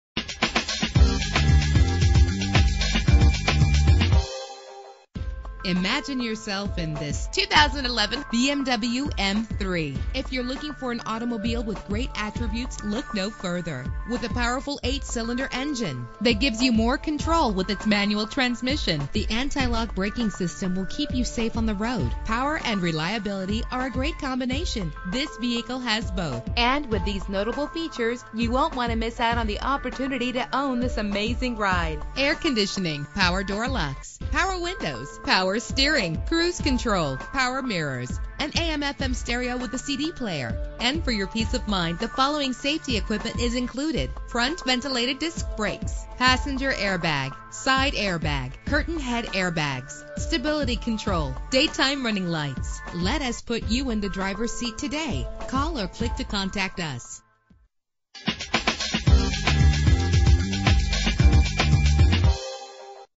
2011 BMW M3 - Chandler AZ
Year: 2011
Make: BMW
Model: M3
Trim: Base
Engine: 4.0 liter 8 cylinder 32 valve
Transmission: Manual
Color: Space Gray Metallic
Mileage:
Address:
7455 W Orchid Lane